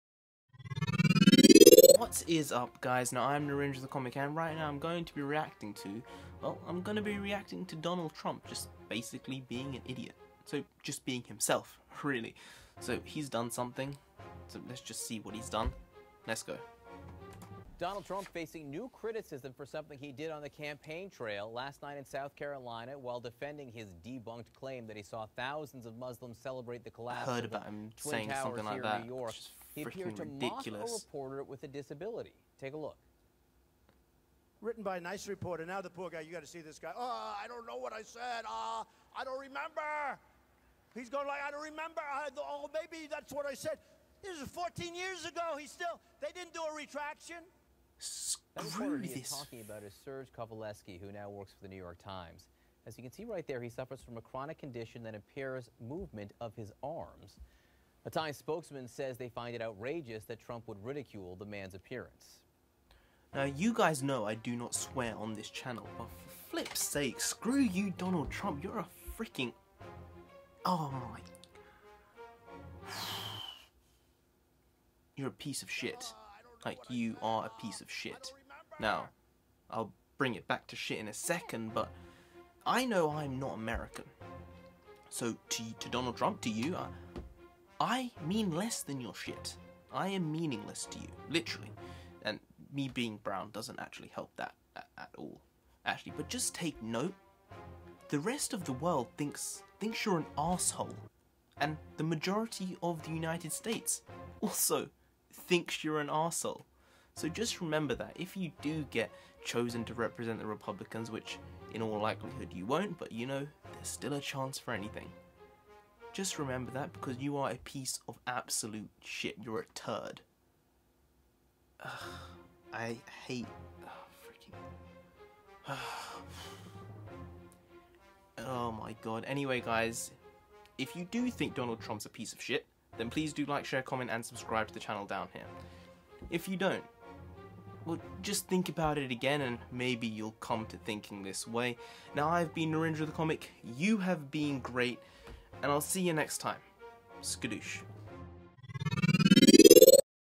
DONALD TRUMP MOCKS DISABLED REPORTER REACTION/RANT
This mother-chucker is just to much... He needs to shut his mouth and keep it that way. This guy boils my blood.